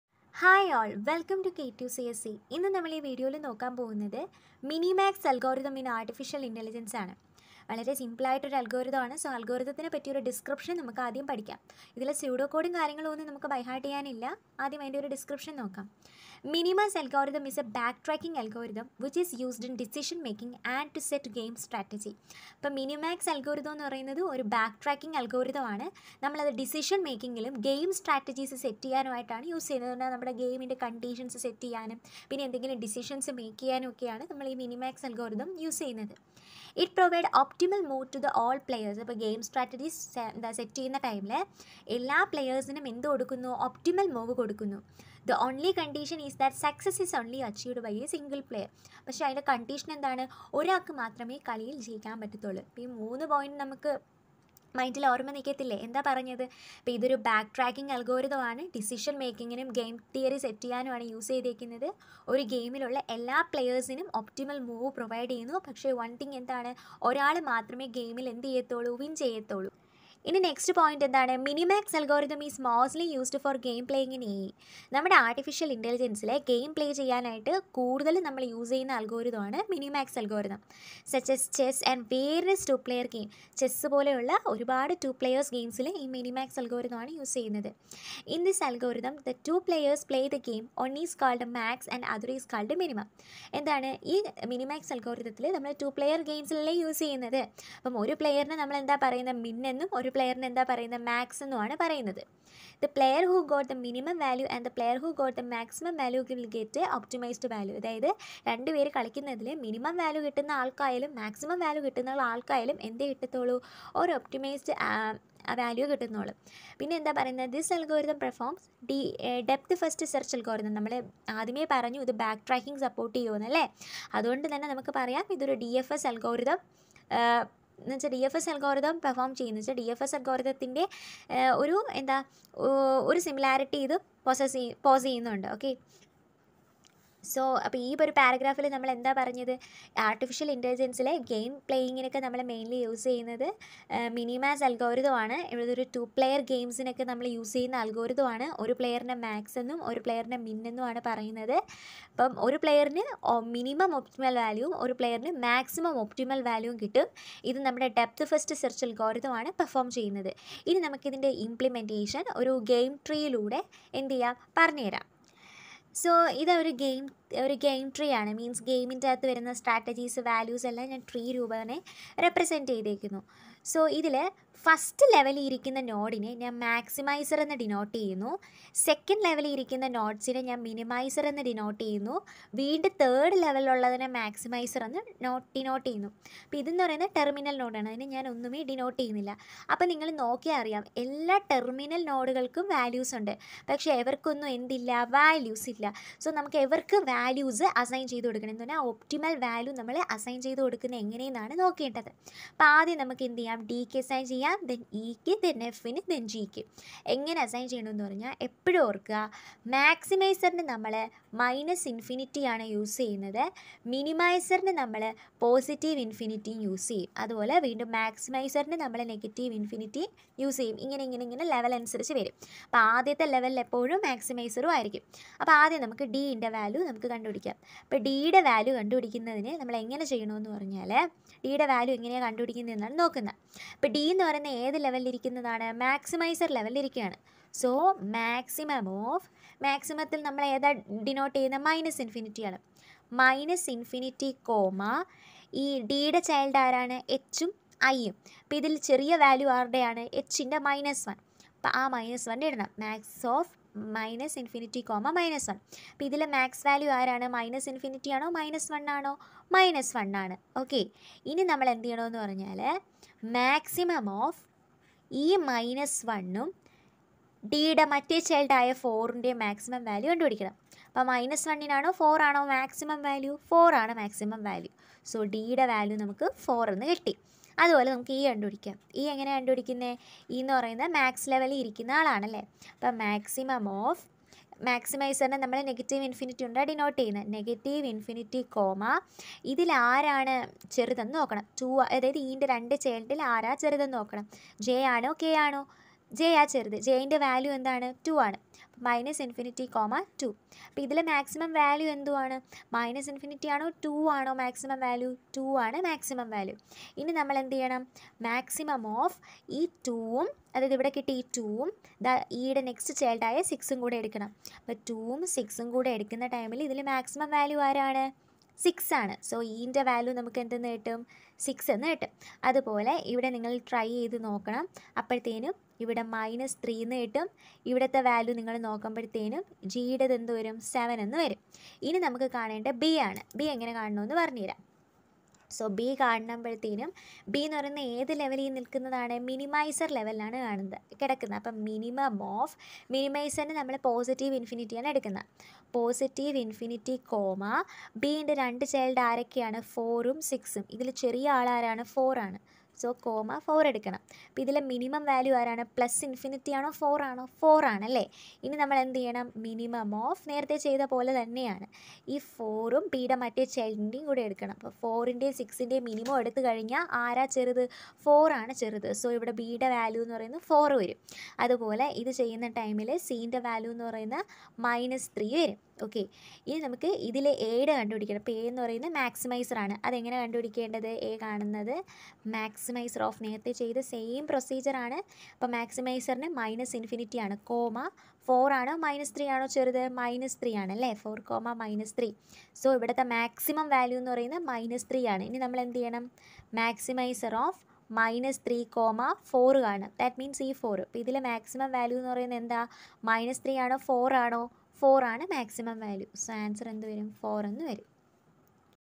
Module 3,Part-2,Mini-Max Algorithm, Artificial intelligence Malayalam video.KTU CSE
Mini-Max Algorithm

S7 CSE, Artificial intelligence Malayalam video KTU 2019 Scheme.